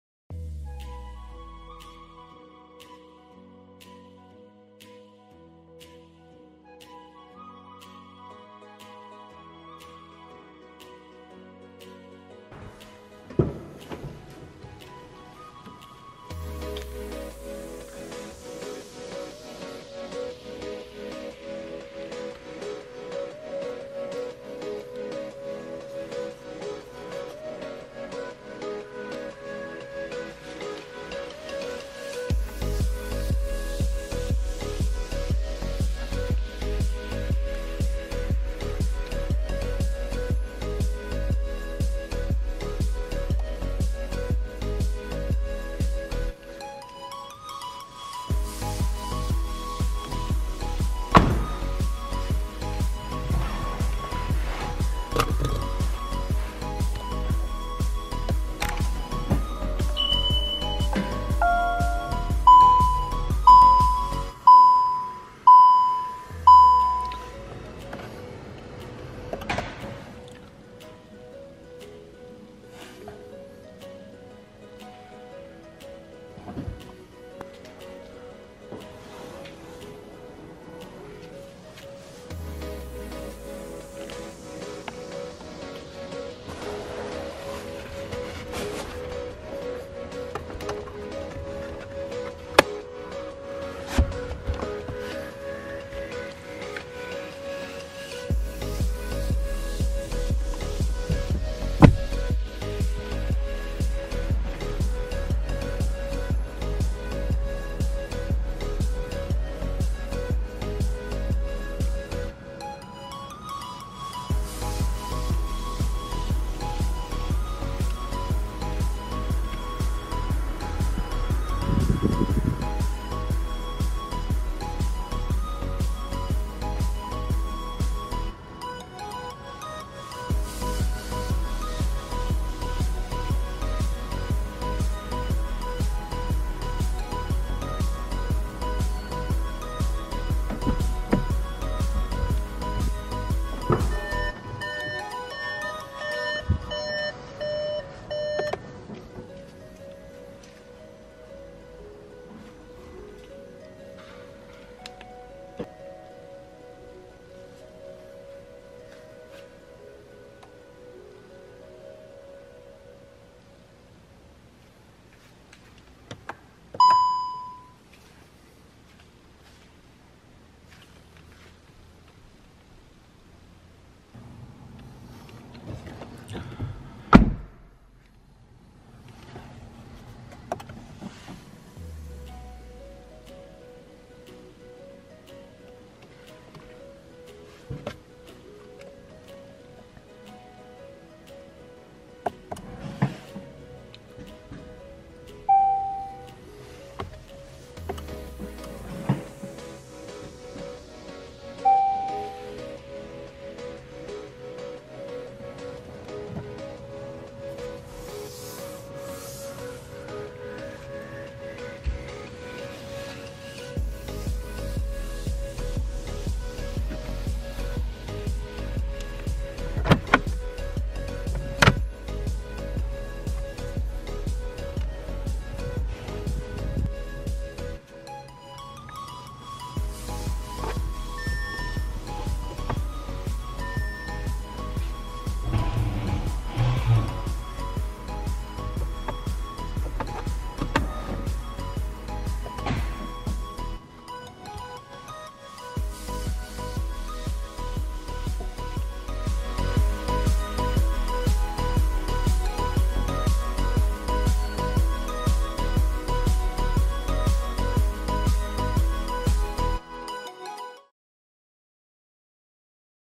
2021 SKODA Octavia Combi RS iV INTERIOR by Supergimm
Taking a quick look at the interior of the new 2021 Skoda Octavia Combi RS iV!Stay tunde,full visual review is coming.